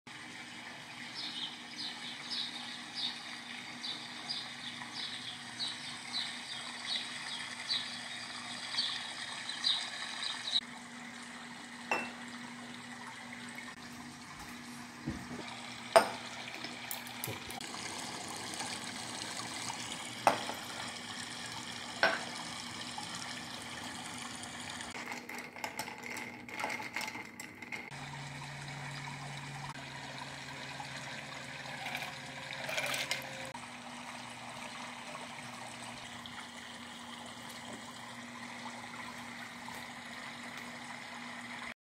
how to make best manhattan#shorts
Stir it for 20 second. best Manhattan that you can have.